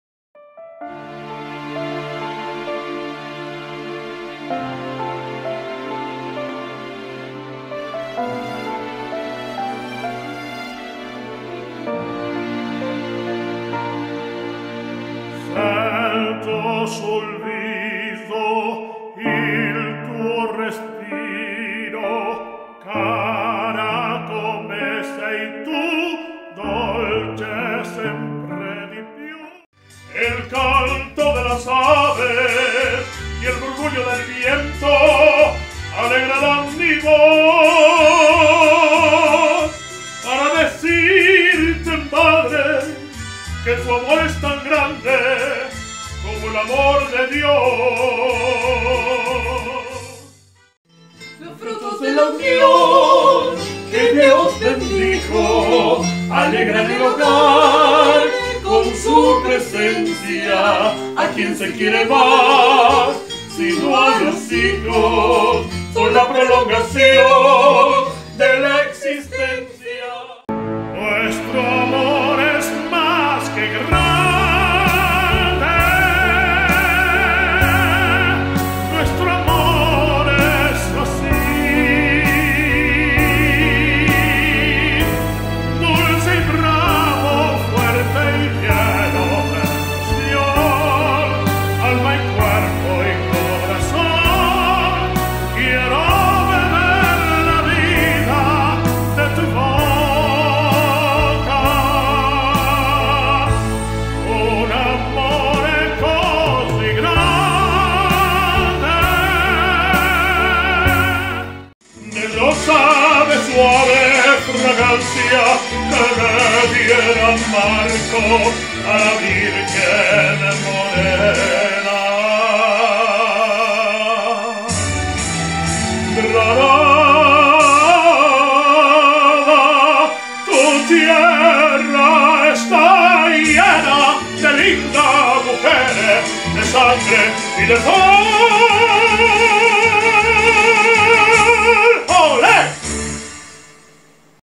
Priolo`s le cantan a Mamá.
Un regalo Musical para el día de las Madres